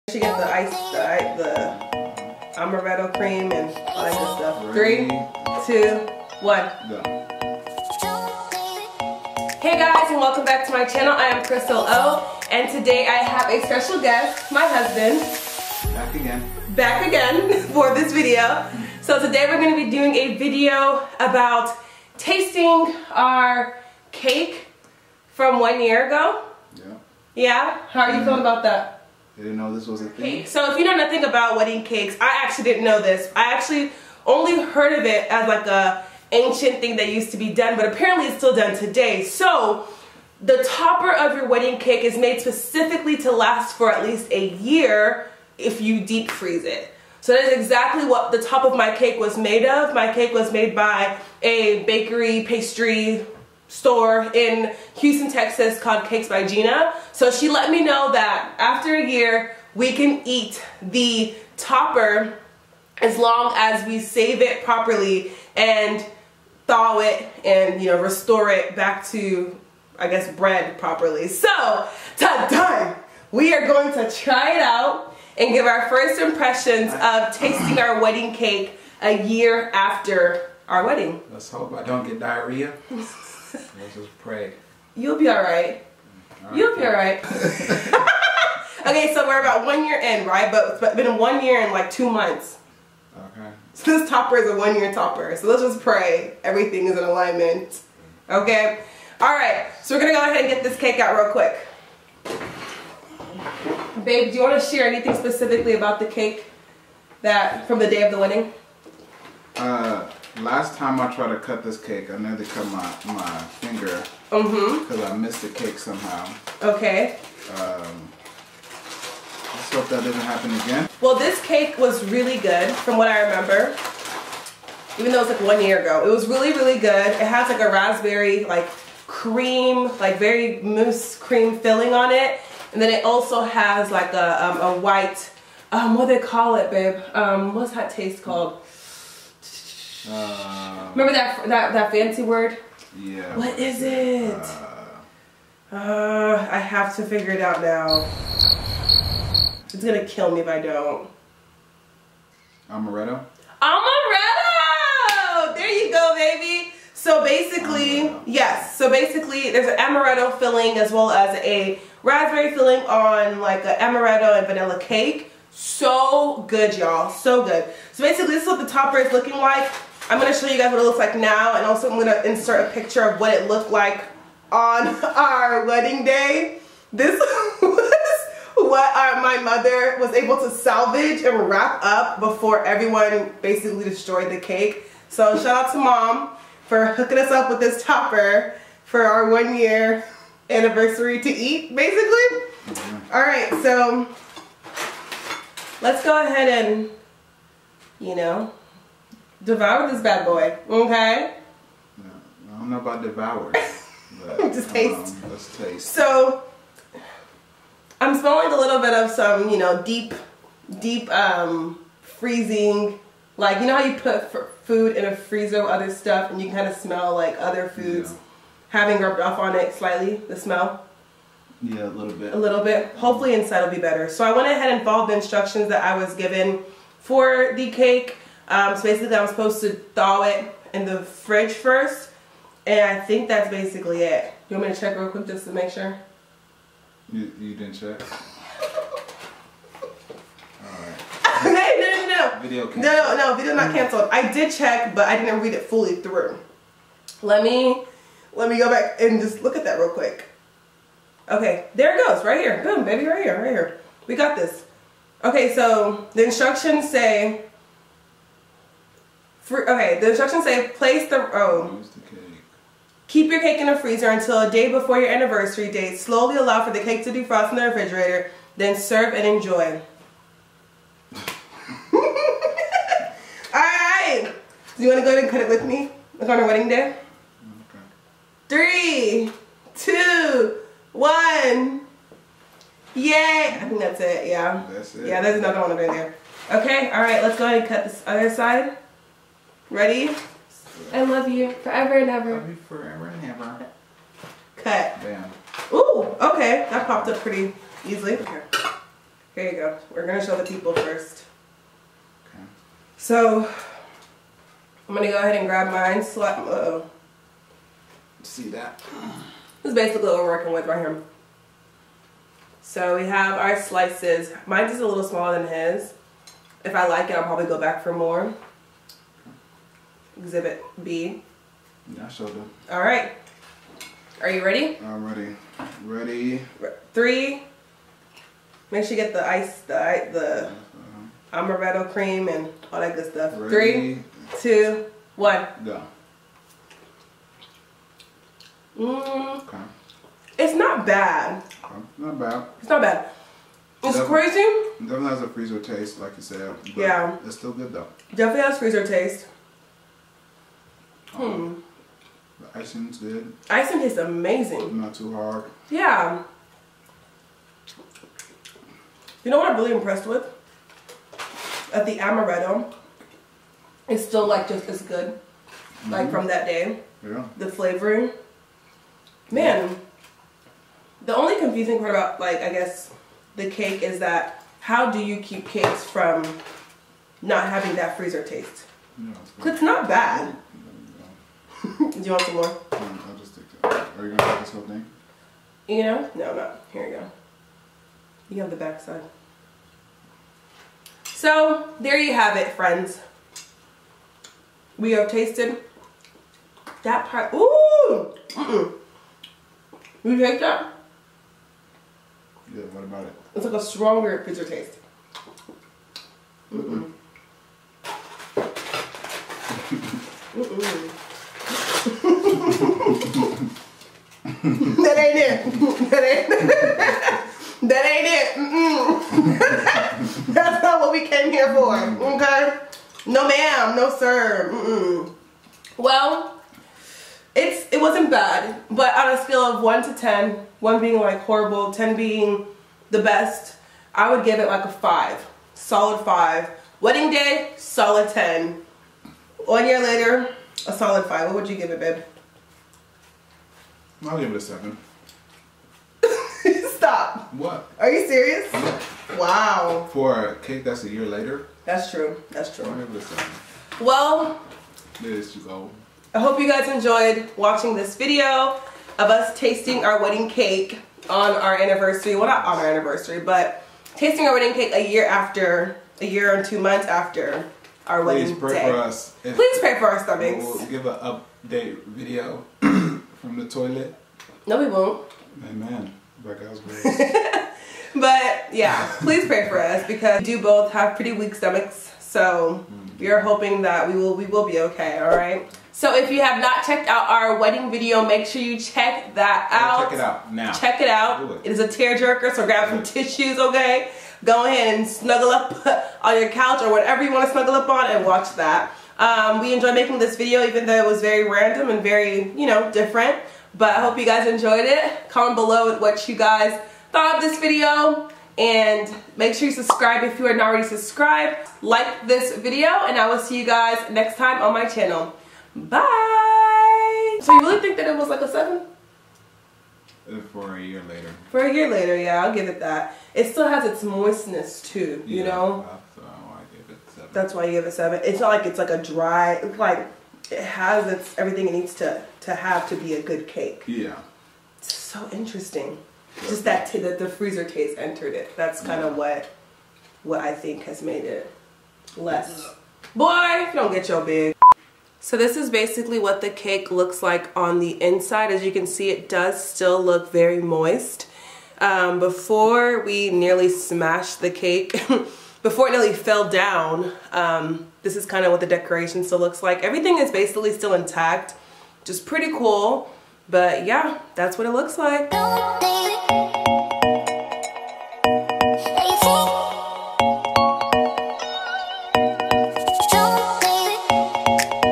We Ate Our Wedding Cake One Year Later And... | CrystalOTv
Hey C-Squad! Today I thought I would record and share our experience with eating our wedding cake a year later. My husband thought I was insane, but here we are. 😂